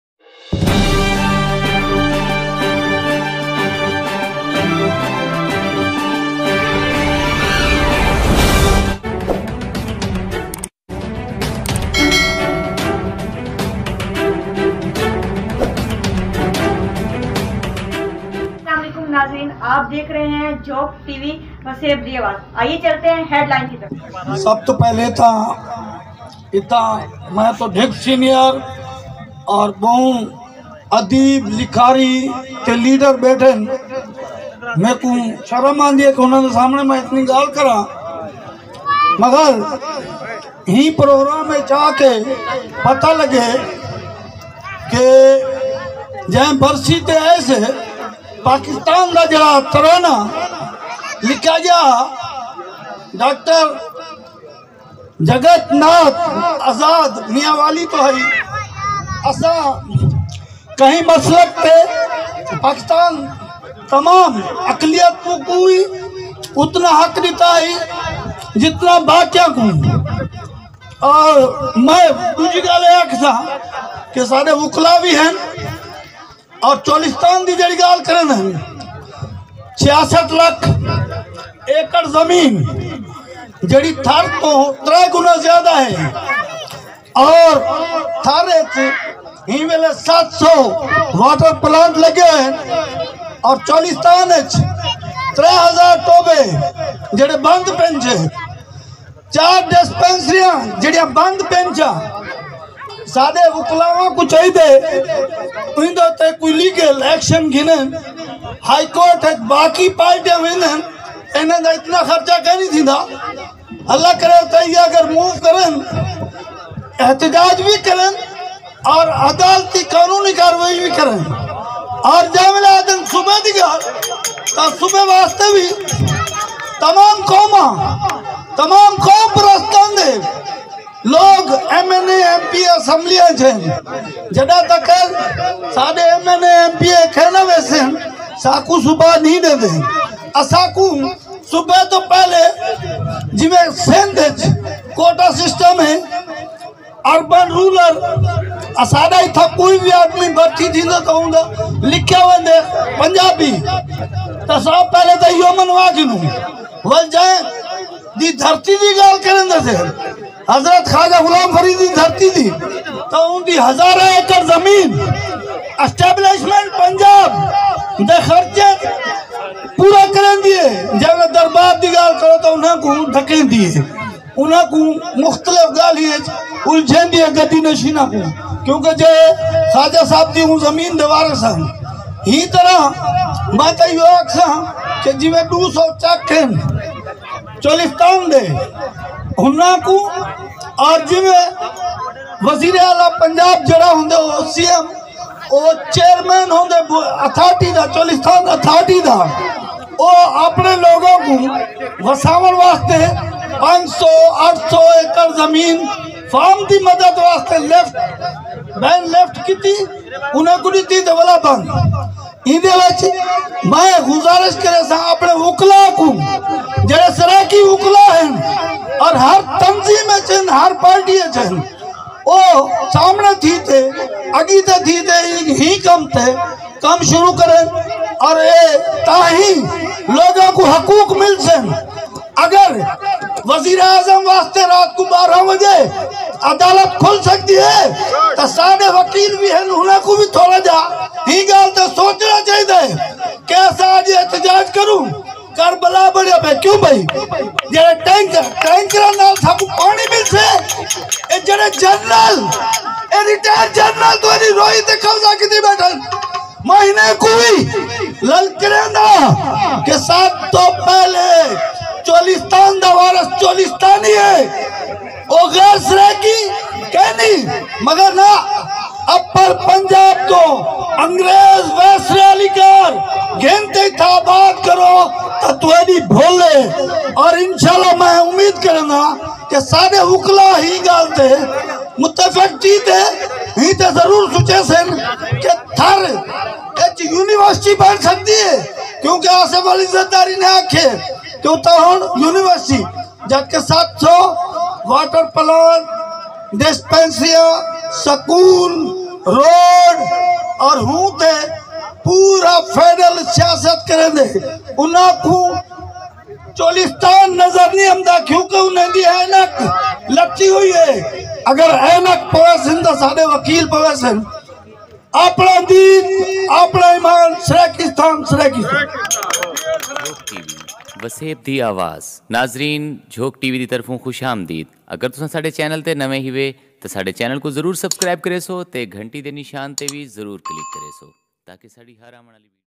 The famous Siraiki lok singer Faqeera Baghat's death Anniversary in Cholistan#facebook google JHOKE
Jhok TV
CEO Rizwan Hussain